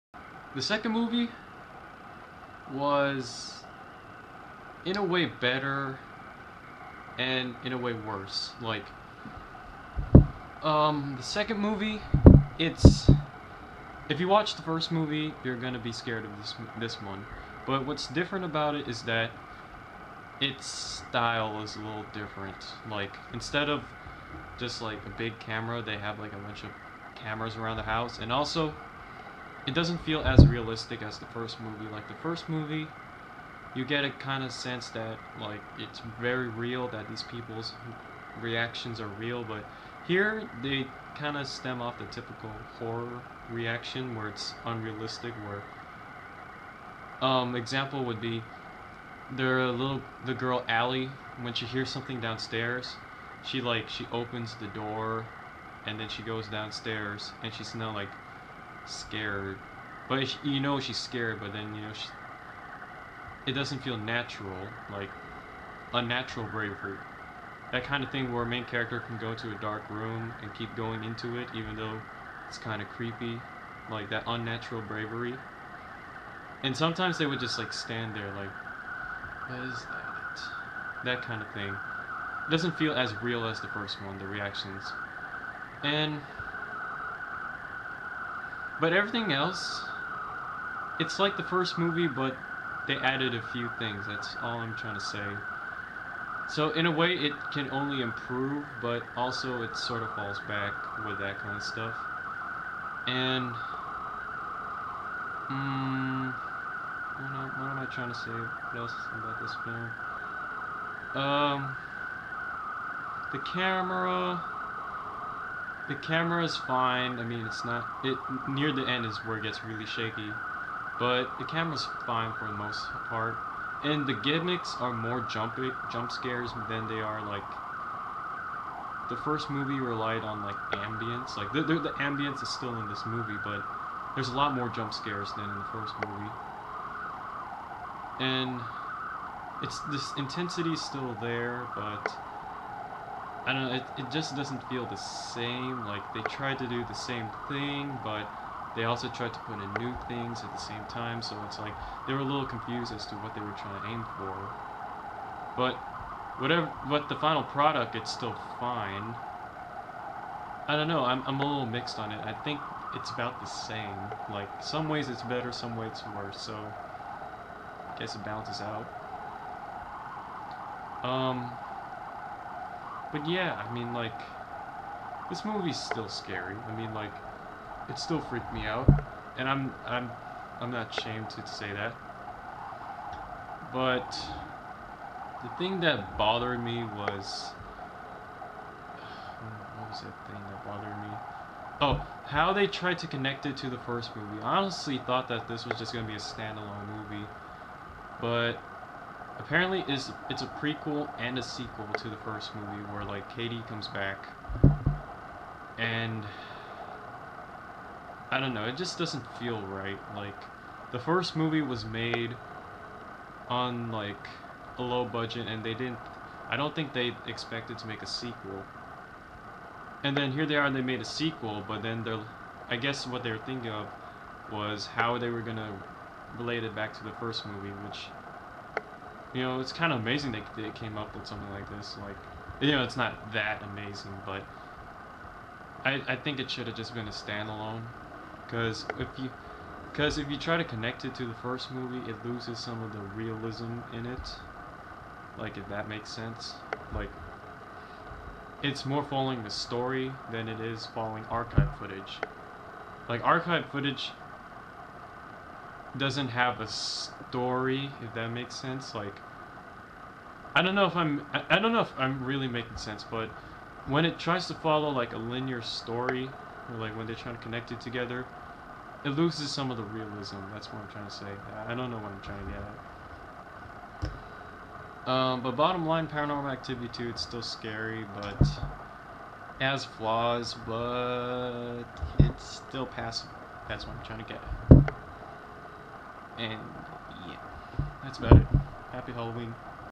"Paranormal Activity 2" Review (2010)
the sequel to one of the scariest movies ever...or is it? well...yes it is.